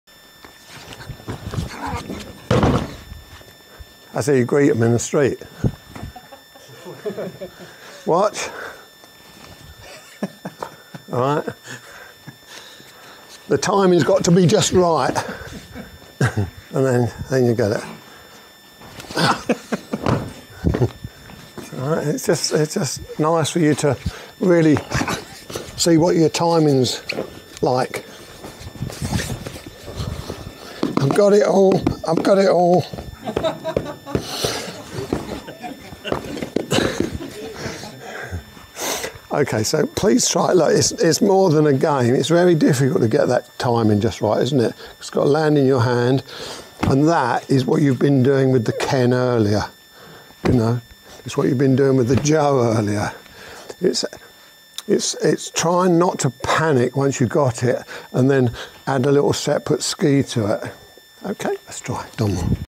Uke being lead by Jo to hand
Shihan Tony Sargeant 7th Dan and Founder of Takemusu Iwama Aikido Europe.
Tony started Aikido in 1973 and continues teaching seminars worldwide.